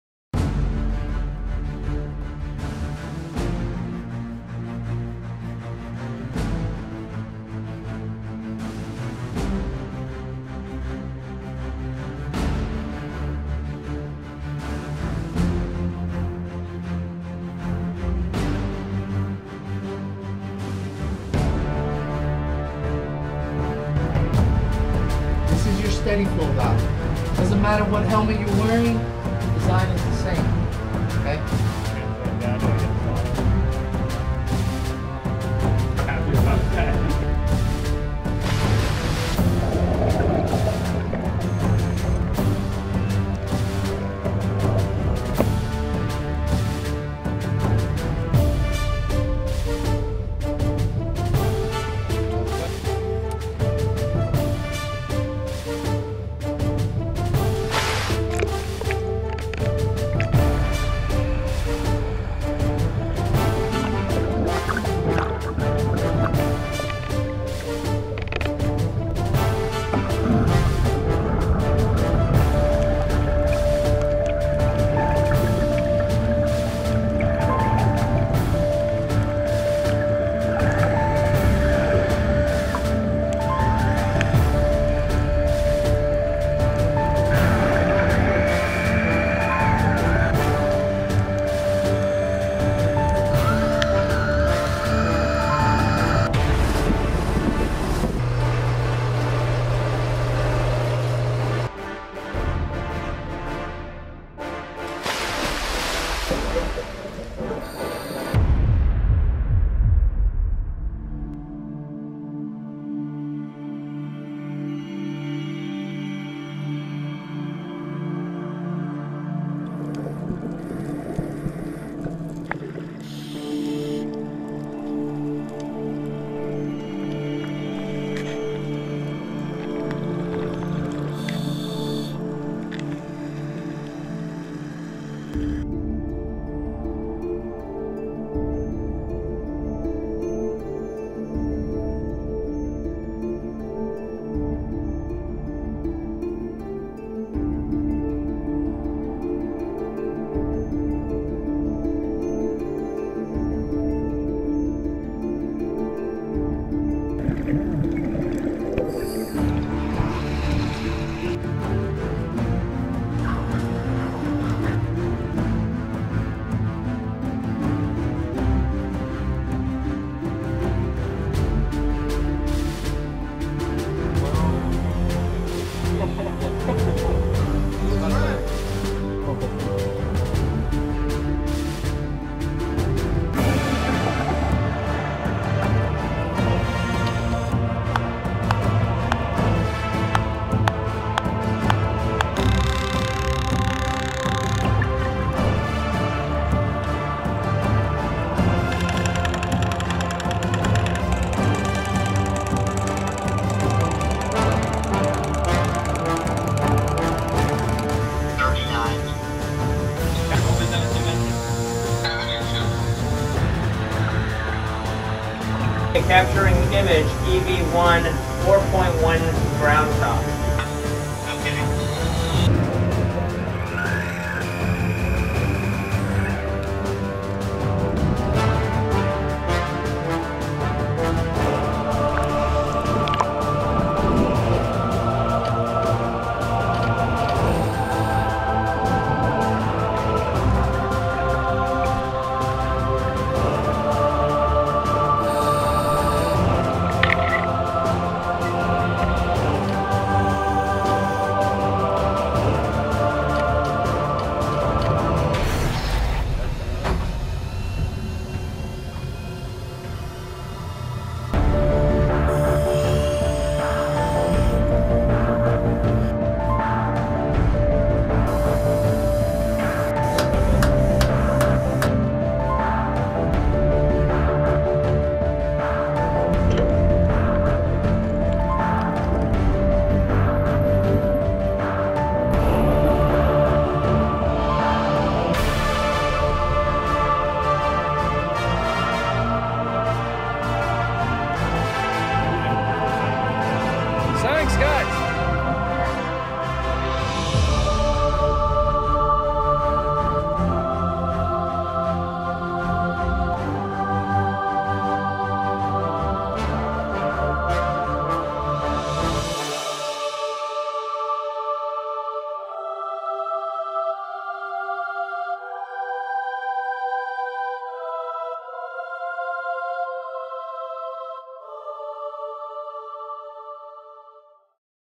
NEEMO 19: SImulating a mission on Mars and its moons
In September 2014, ESA astronaut Andreas Mogensen and ESA astronaut trainer and spacewalk instructor Hervé Stevenin joined an international crew for a seven-day underwater mission for NEEMO – NASA Extreme Environment Mission Operations.

NEEMO’s underwater habitat off Florida acts as makeshift a space base for astronauts to make regular ‘waterwalks’ in full scuba gear. Both underwater missions plan sorties for the astronauts to simulate spacewalks.

Their mission focused on wearable computers and remotely monitoring the crew. An ESA voice-command ‘mobiPV’ prototype using smartphones, tablets and screen glasses was tested. This approach was later used on Andreas’ space mission in September 2015.